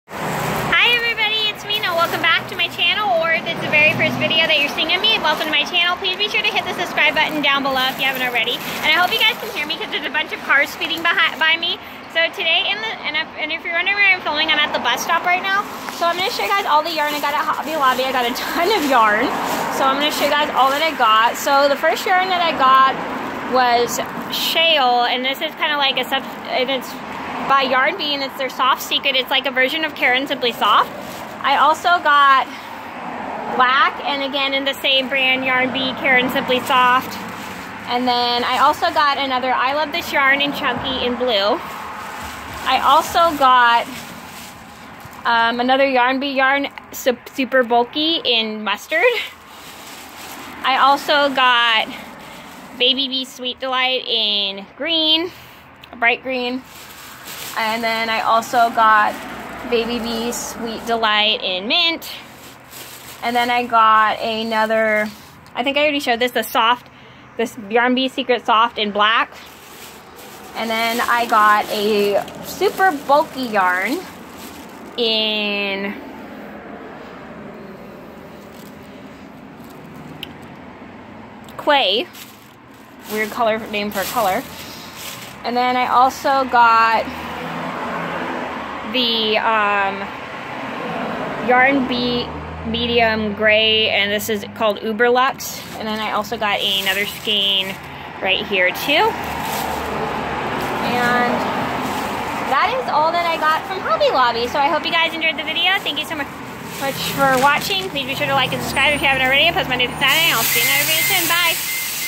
What Mena Got At Hobby Lobby # 3
Just showing everyone what I bought at Hobby Lobby.